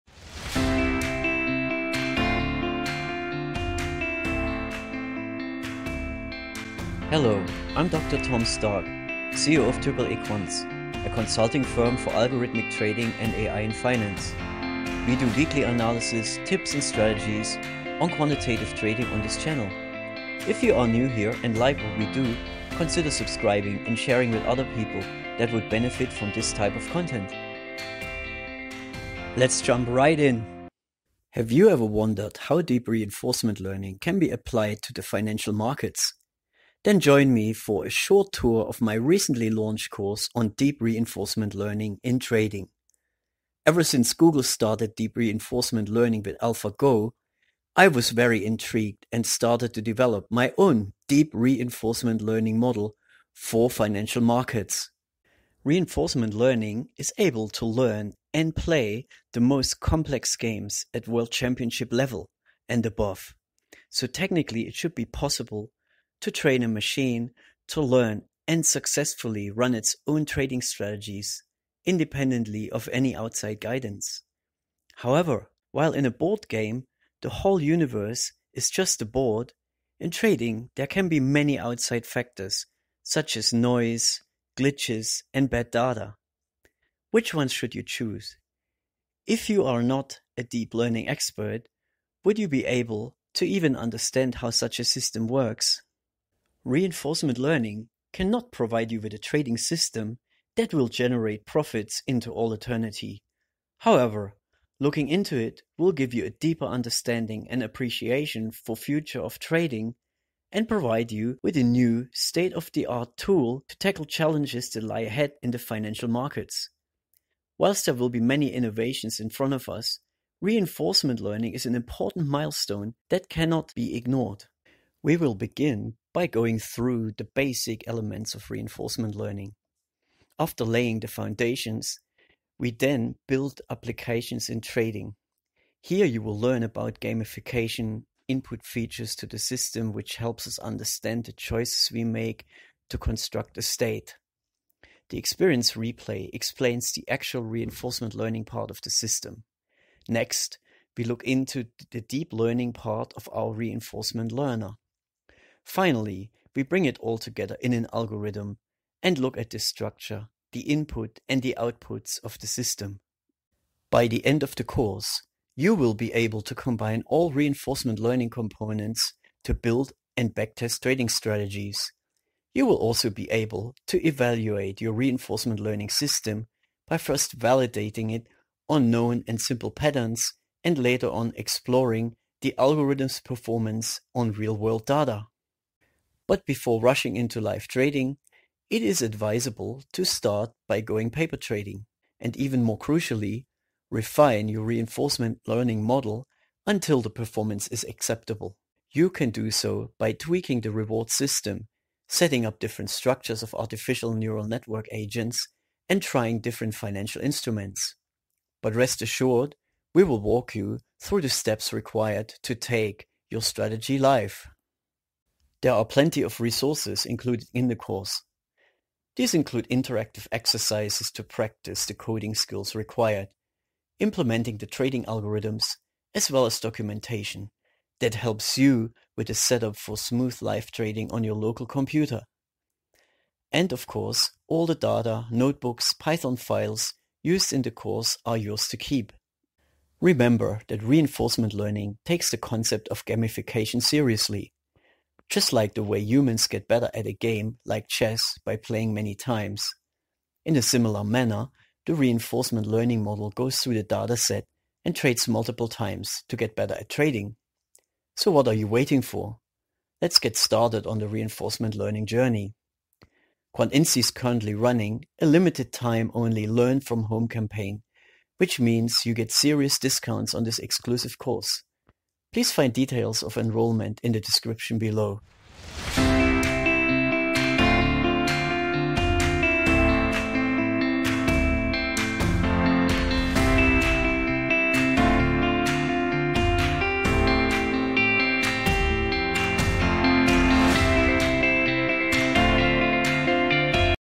Deep Reinforcement Learning in Trading | Course Announcement
Ever since Google started Deep Reinforcement Learning with AlphaGo, I was very intrigued and started to develop my own deep reinforcement model for financial markets. Please join me for a short tour of my recently launched course on ‘deep reinforcement Learning in Trading’.

In this course, you will have a close look at deep reinforcement learning and build a system that turns trading in the financial markets into a game that can be learned by a machine to make profitable trading decisions.